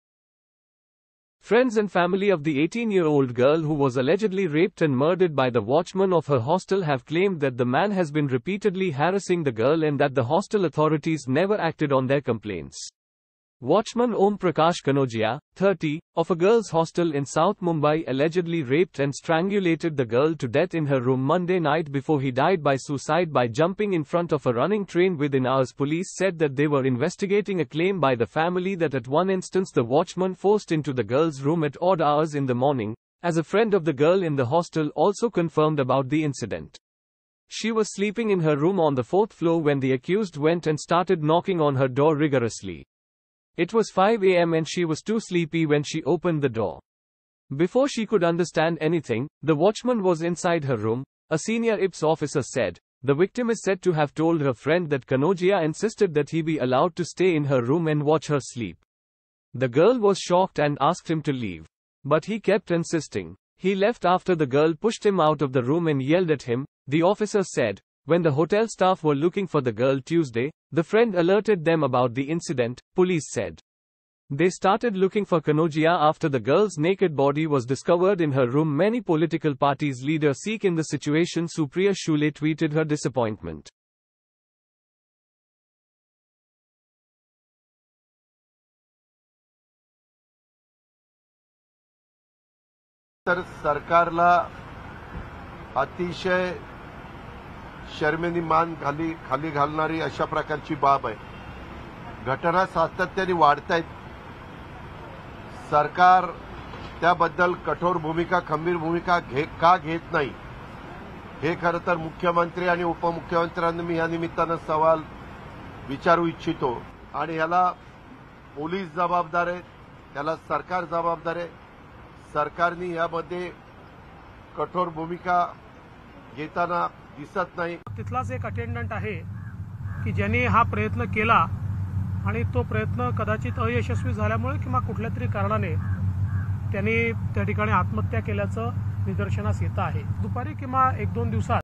Girl are not safe in girls hostel | a shameful act took place in Marine line
Voice Over:
Video Editor: KAJAL TIWARI